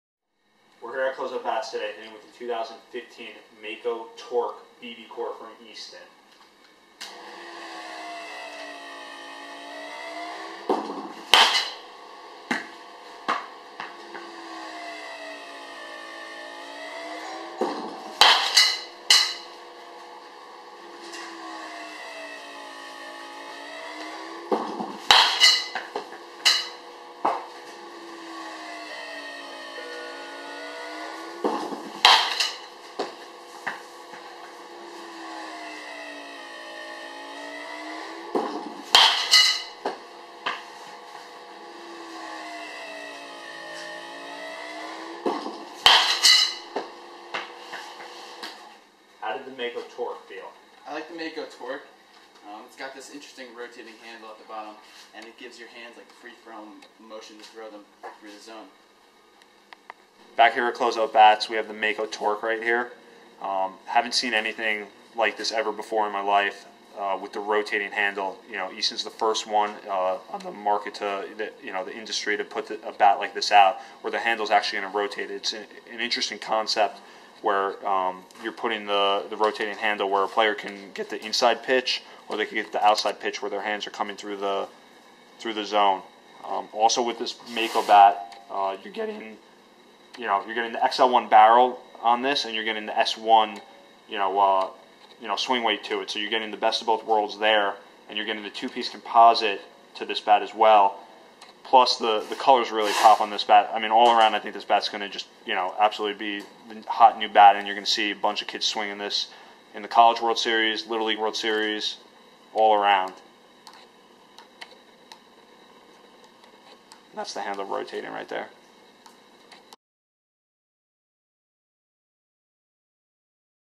2015 Easton Mako BB15MKT Torq BBCOR Tech closeoutbats.com
Turk comes by and crushes with the brand new Mako Torq. The handle on this bat will actual rotate. The bat will be available in to buy in September.